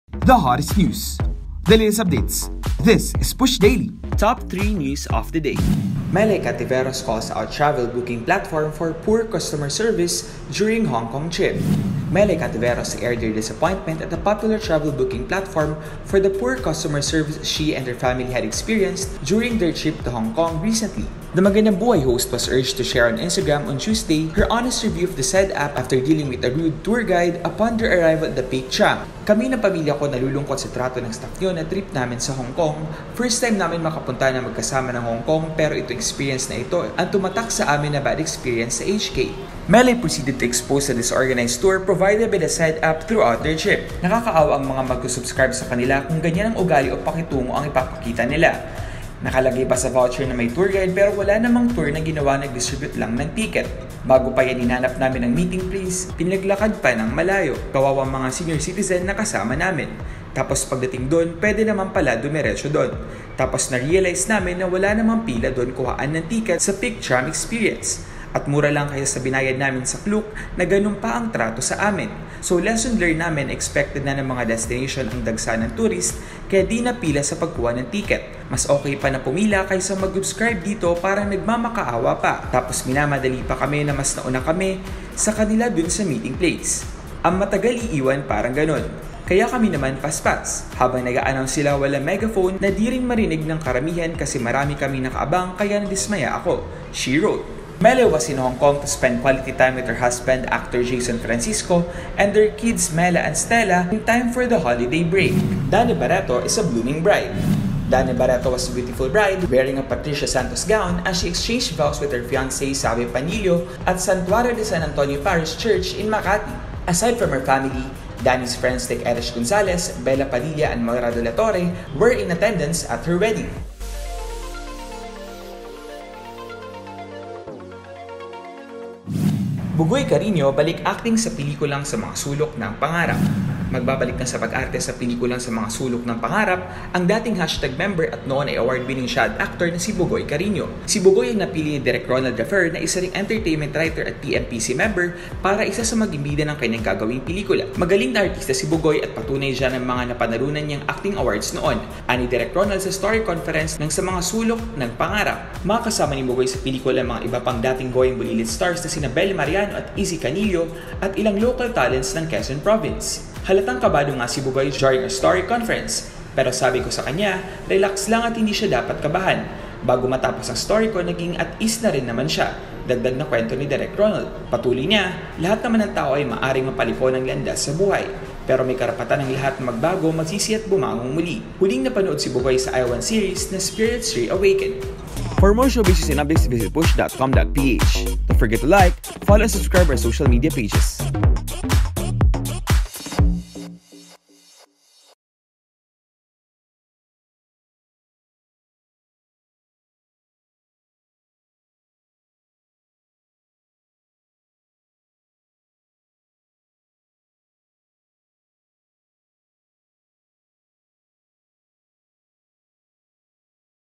Melai Cantiveros, Dani Barretto and Bugoy Carino | Push Daily Top 3
PushDaily Top 3: Melai Cantiveros calls out travel booking platform for poor customer service during Hong Kong trip. Dani Barretto is a blooming bride. Bugoy Carino, balik-acting sa pelikulang 'Sa mga sulok ng pangarap.'

For the hottest news and latest showbiz updates, visit push.abs-cbn.com